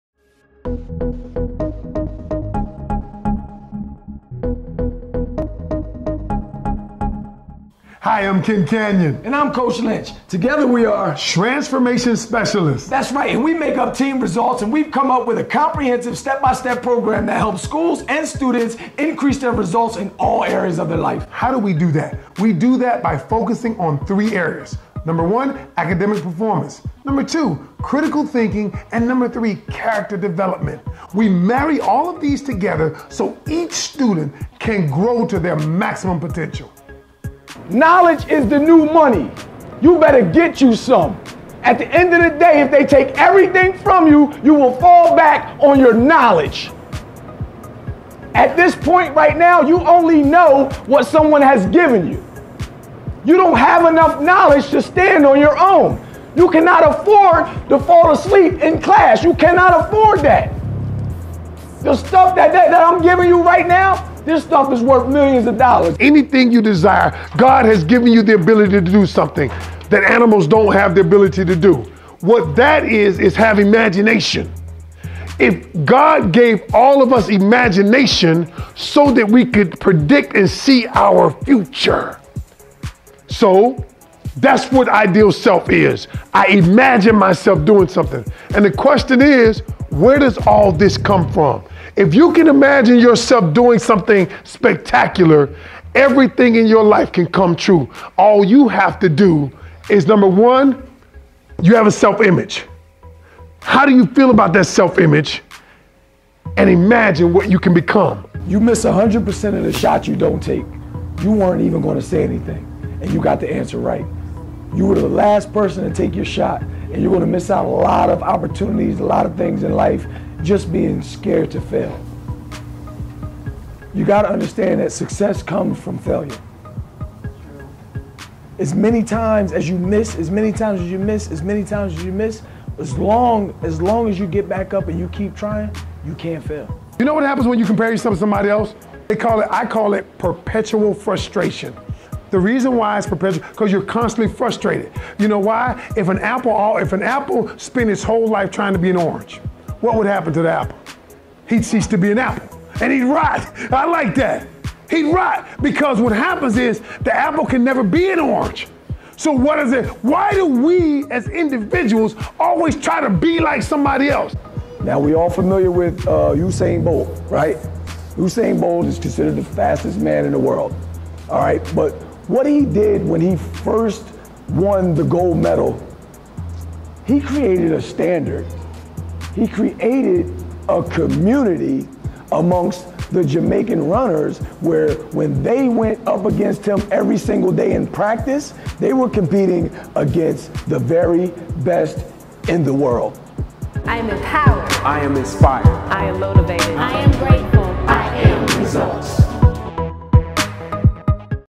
School of Resultz- Schools Development Program
A comprehensive Student Development Program tailored to your specific needs. It is not a one-size-fits-all program nor a scope & sequence curriculum.Instead, our strategy focuses on the root of change – mindset & behavior modification. Our framework helps develop Academic, Social, Emotional skills and Character traits.
Properly implemented, our program becomes infused seamlessly into the DNA of your school or organizational climate. Implementation can occur at your own pace and can range in complexity based upon your needs and goals.